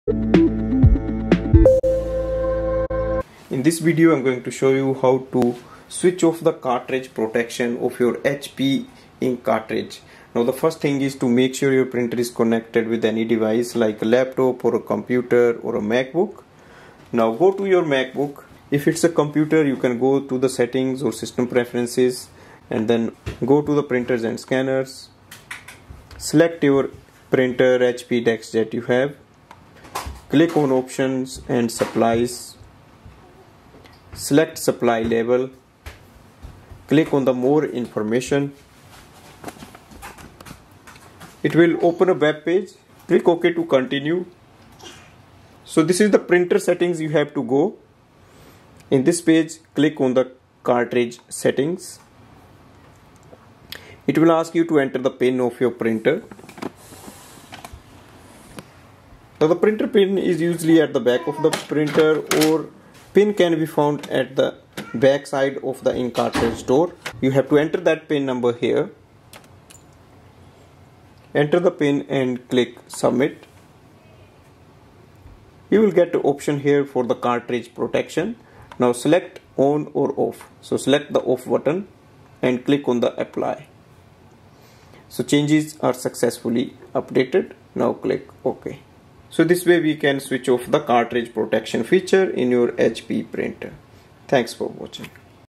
Hp Cartridge Protection Remove, Disable, Turn off, Reset or Turn On To Use in Other Printer.
This video reviews the method to disable or remove or reset or turn off the cartridge protection of your HP DeskJet Printer. This helps in HP DeskJet 2710, 2710e, 2720, 2720e, 2721, 2721e, 2722, 2722e, 2723, 2723e, 2724, 2732, 2742e, 2752, 2752e, 2755, 2755e, 4110e, 4120e, 4121e, 4122e, 4123e, 4130e, 4132e, 4152e, 4155e, 4158e, DeskJet Ink Advantage 2775, 2776, 4175, 4176, DeskJet Plus 4110, 4120, 4121, 4122, 4130, 4140, 4152, 4155, 4158, DeskJet Ultra Ink Advantage 4825, 4826, 4828, 4829, and 4877 All-in-One printers.